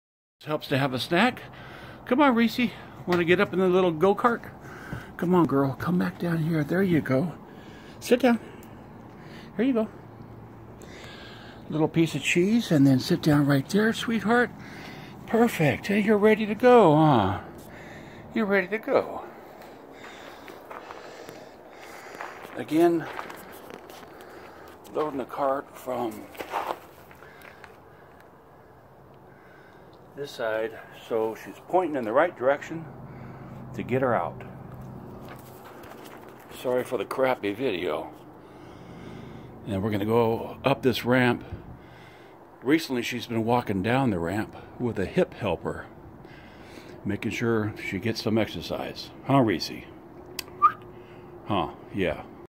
Reese - transporting Injured dog; loading in wagon
DIY Home Depot wagon converted to transport Injured Dog